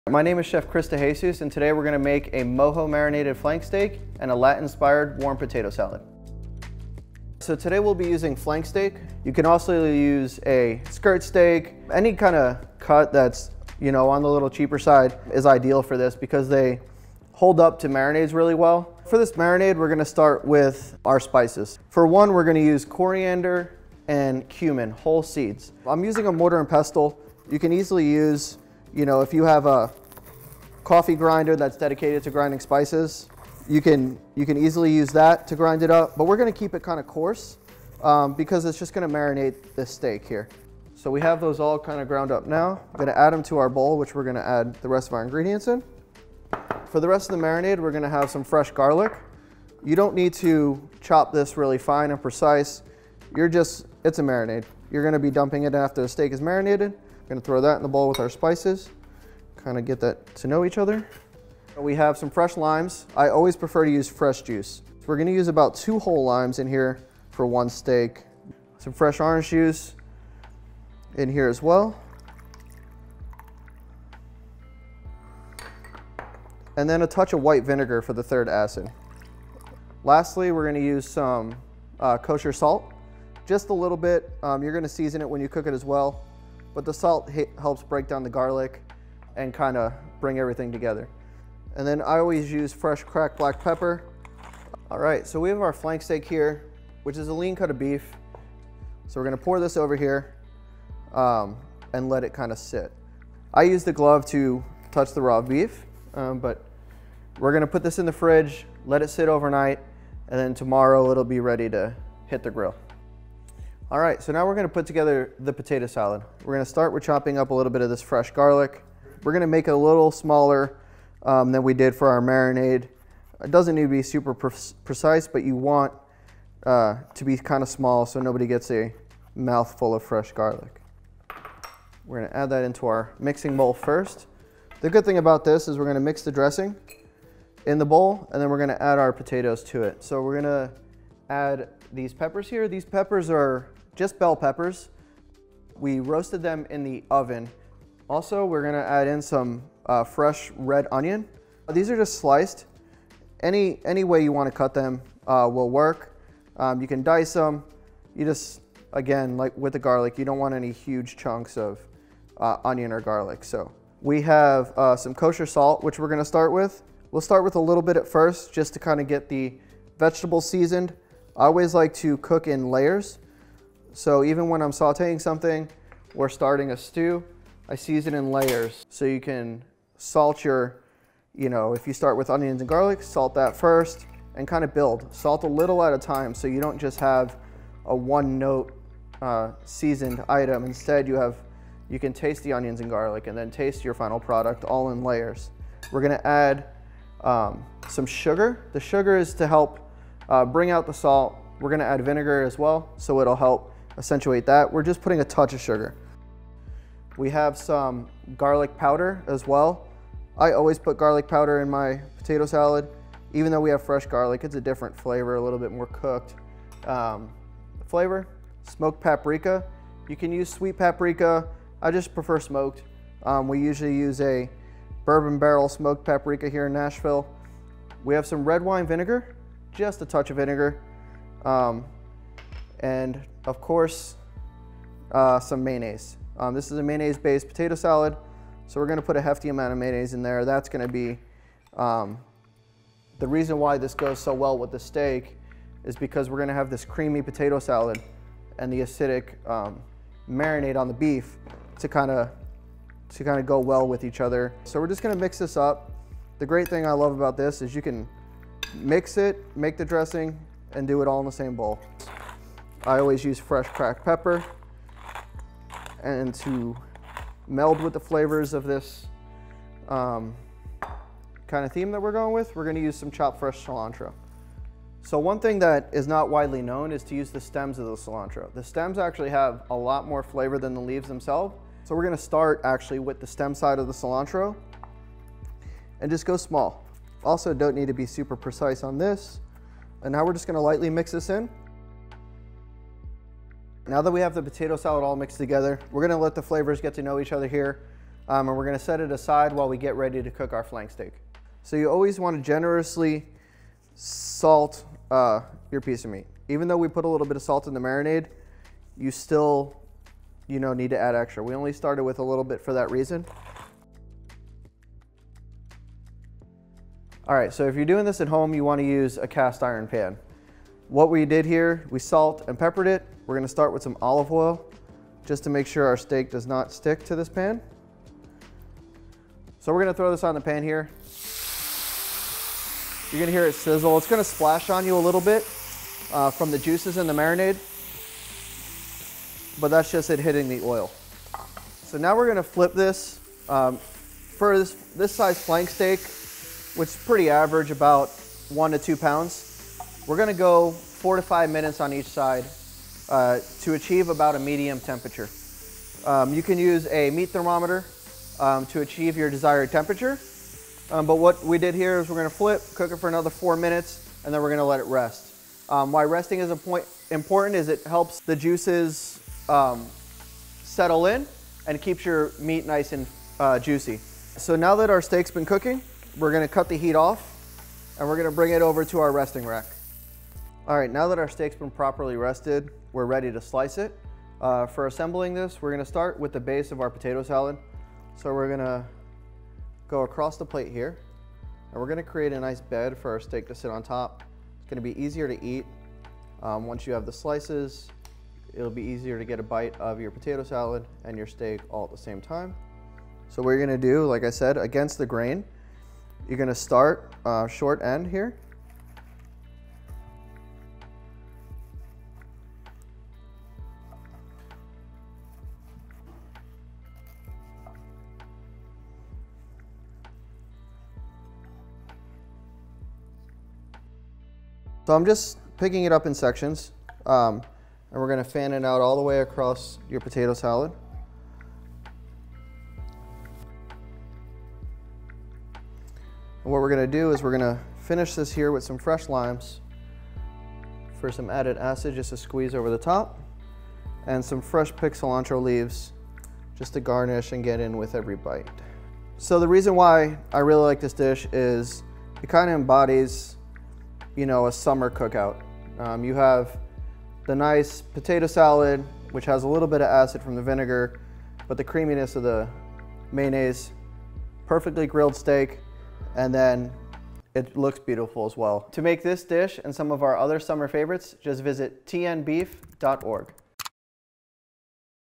Mojo Marinated Flank Steak & Potato Salad
Chef Chris de Jesus shares a twist on Cuban-style grilled beef using traditional flavors of Mojo to flavor and tenderize the flank steak before grilling.